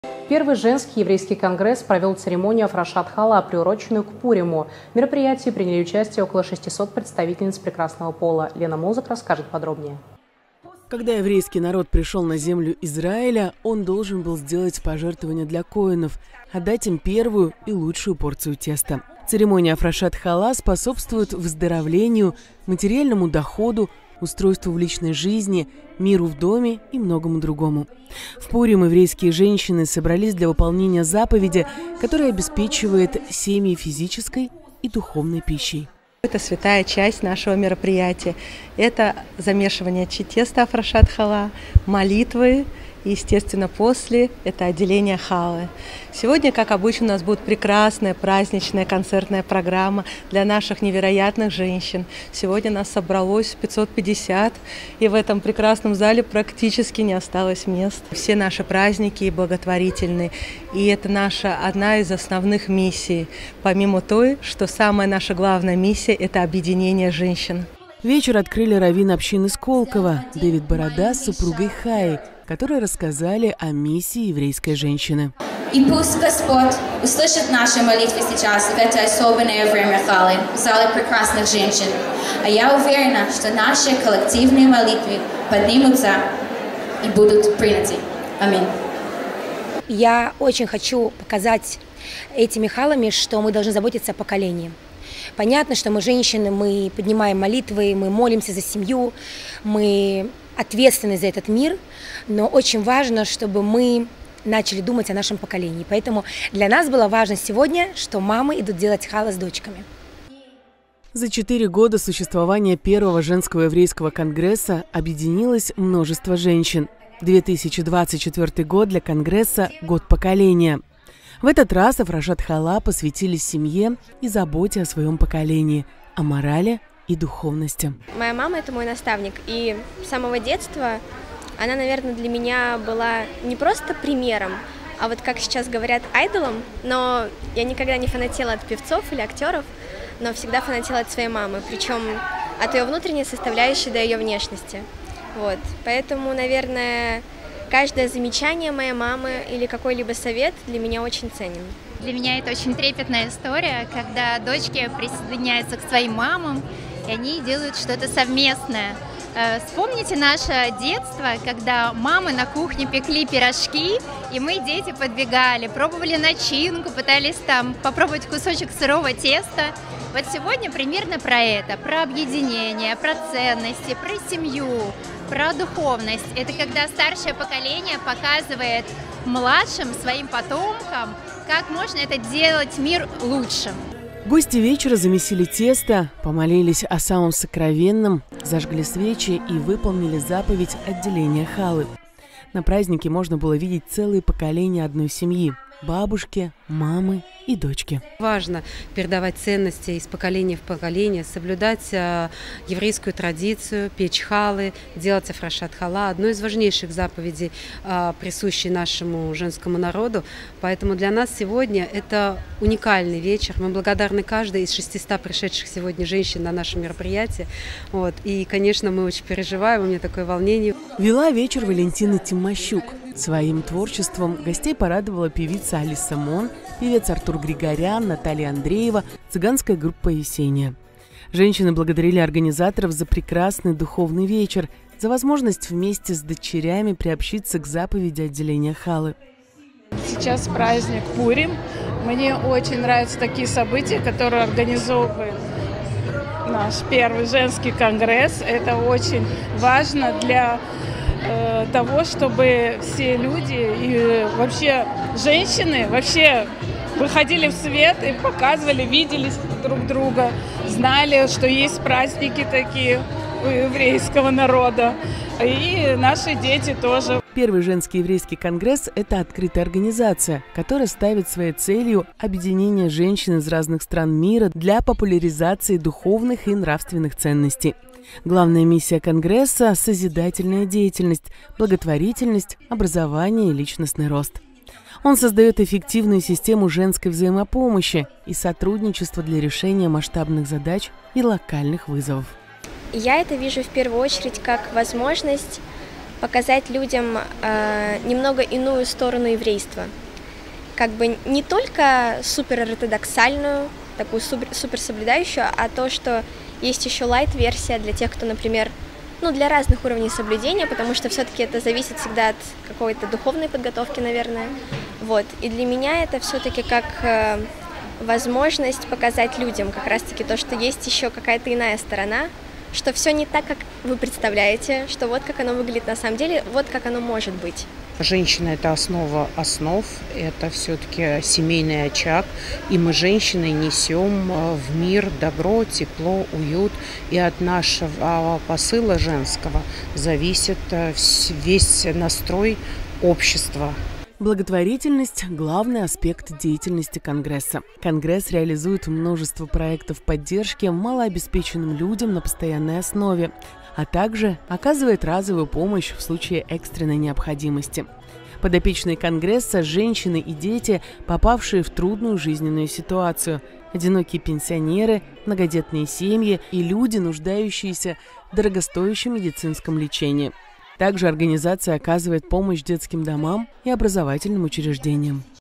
Афрашат Хала: «Еврейская женщина - свеча мира»
Первый женский еврейский конгресс провёл церемонию афрашат хала. В мероприятии приняли участие около шестисот представительниц прекрасного пола еврейского происхождения.